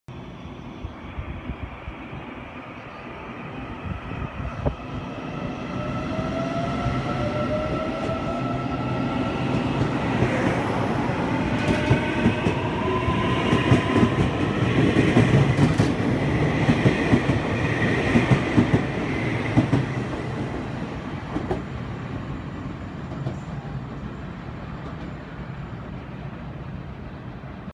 109年12月12日1218次小小兵中壢出站
1218 經由  山線  苗栗→基隆
(苗栗端)ED891+ED892(基隆端)
本次列車沿途停靠
苗栗  到  基隆  區間中途個站皆停